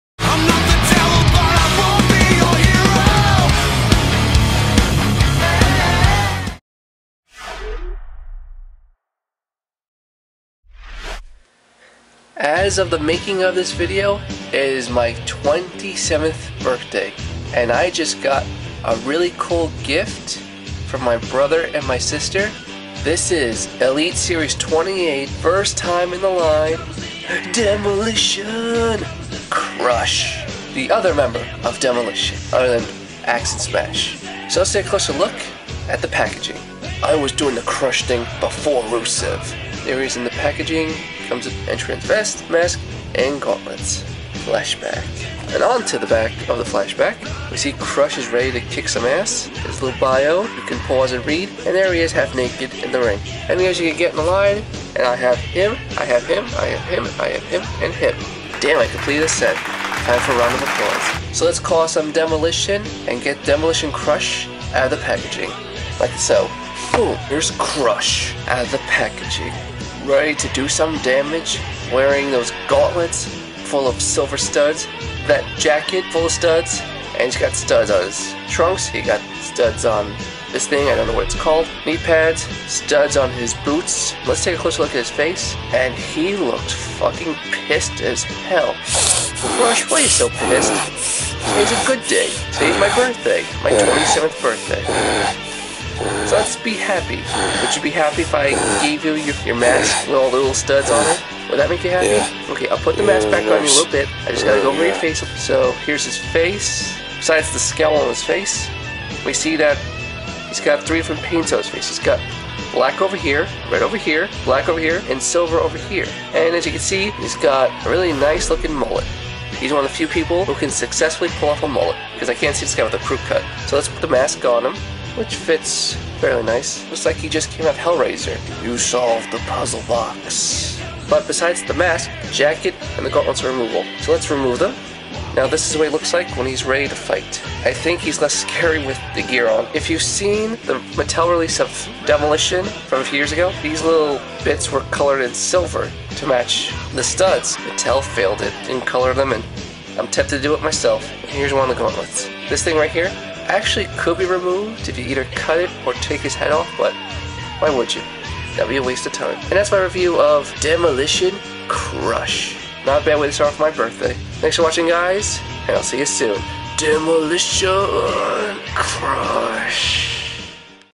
My WWE Figures:Demolition Crush Mattel Elite Series 28 Flashback
I was,genuinely,surprised when my brother and sister chipped in and bought my Elite Series 28 Flashback,Demolition,Crush.
It's a really cool figure,even though,there is paint missing on his jacket,why Mattel,why?! RIP Crush!